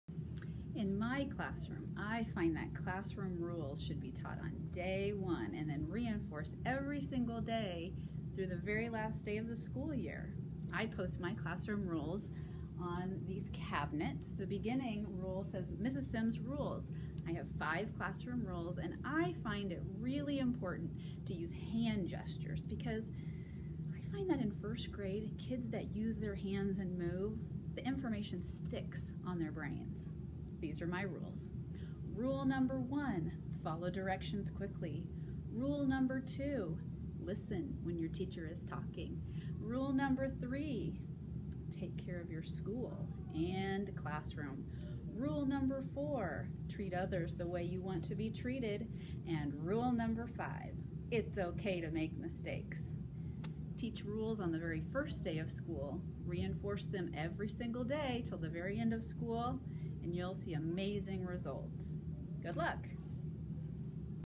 Classroom Rules
Jena Simms | Wichita Collegiate School | Wichita, Kansas

Use hand gestures to reinforce classroom rules starting on day one, and repeat them often on a daily basis.